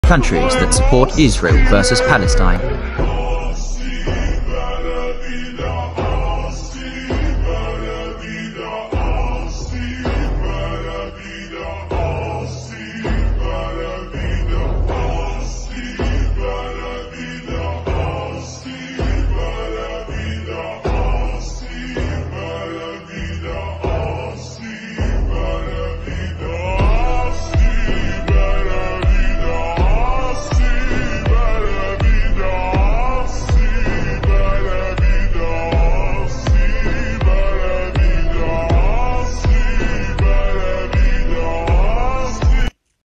Countries that support Israel 🇮🇱 Vs Palestine 🇵🇸
Hey guys I’m back with another video it’s countries that support Israel Vs Palestine it is done by political relations nothing to do with people.